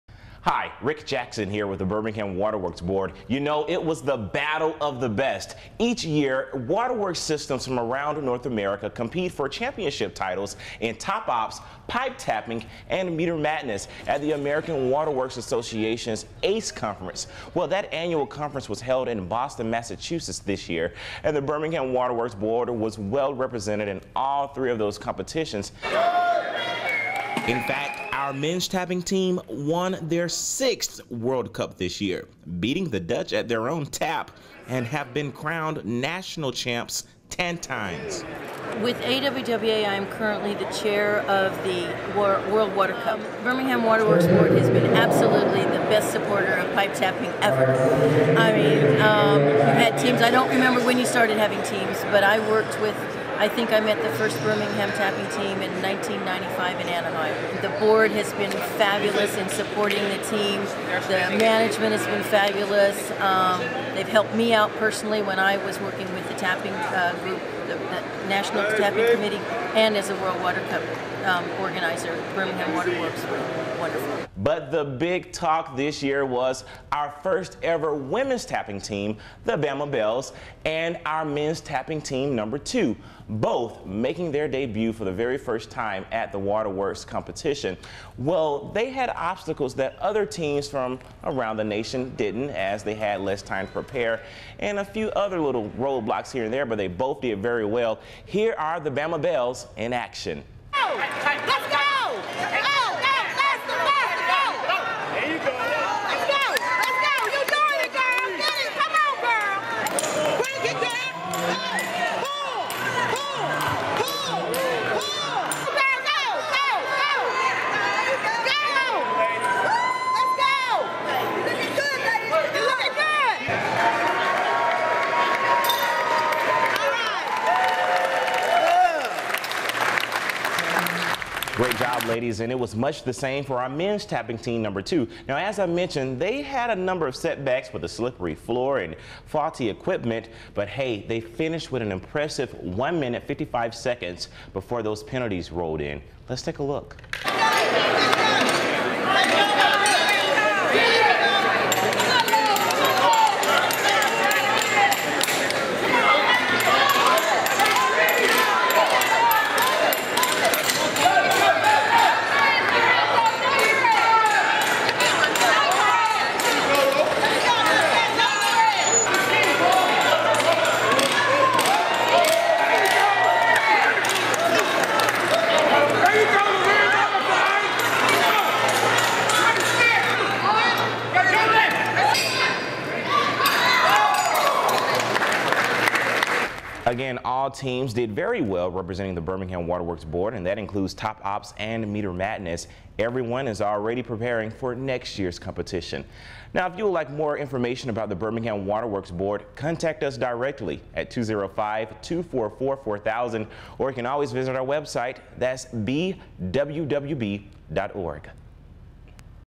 AWWA ACE 2014
BWWB's Bama Belles and Men's Tapping Team #2 made their debut at AWWA's national pipe tapping competition in Boston, MA. Our Men's Tapping Team brought home the second place title!